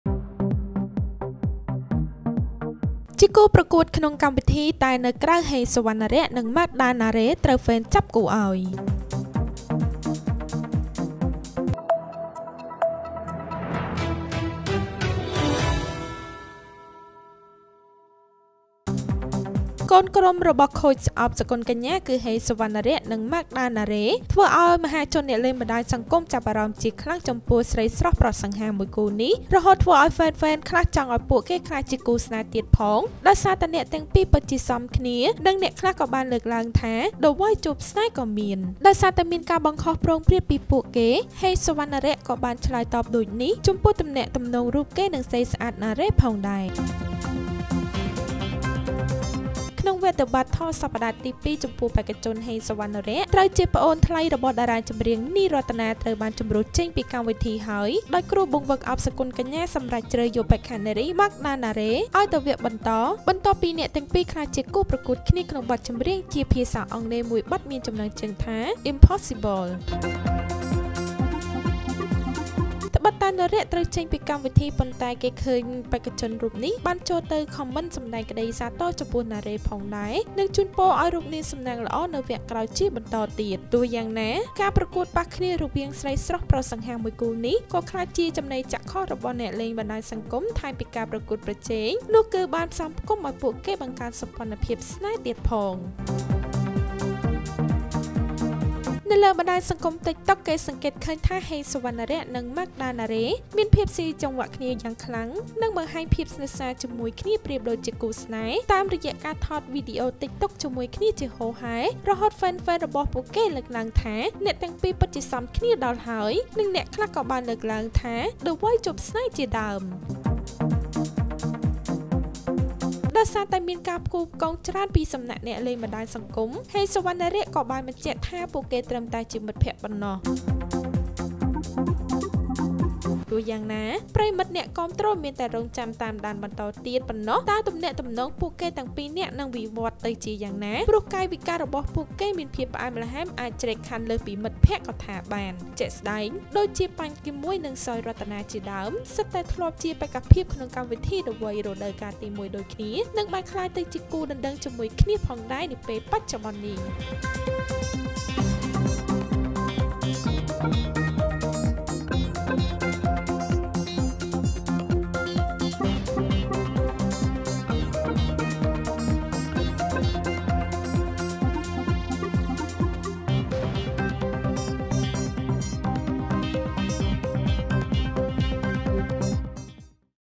ជាគូប្រកួតក្នុងកម្មវិធីតែនៅក្រៅ ហេង សុវណ្ណនរៈ និង ម៉ាក្ស ដាណារ៉េ ត្រូវហ្វេនចាប់គូឱ្យ
កូនក្រុមរបស់ Coach ឱក សុគន្ធកញ្ញា គឺ ហេង សុវណ្ណនរៈ និង  ម៉ាក្ស ដាណារ៉េ ធ្វើឱ្យមហាជនអ្នកលេងបណ្តាញសង្គមចាប់អារម្មណ៍ជាខ្លាំងចំពោះស្រីស្រស់ប្រុសសង្ហាមួយគូនេះ រហូតធ្វើឱ្យហ្វេនៗខ្លះចង់ឱ្យពួកគេក្លាយជាគូស្នេហ៍ទៀតផងដោយសារអ្នកទាំងពីរពិតជាសមគ្នា និងអ្នកខ្លះក៏បានលើកឡើងថា The Voice ជួបស្នេហ៍ក៏មាន។ ដោយសារតែមានការបង្ហោះព្រោងព្រាតពីពួកគេ ហេង សុវណ្ណនរៈ ក៏បានឆ្លើយតបដូចនេះចំពោះទំនាក់ទំនងរូបគេនិង ស្រីស្អាត ណារ៉េ ផងដែរ។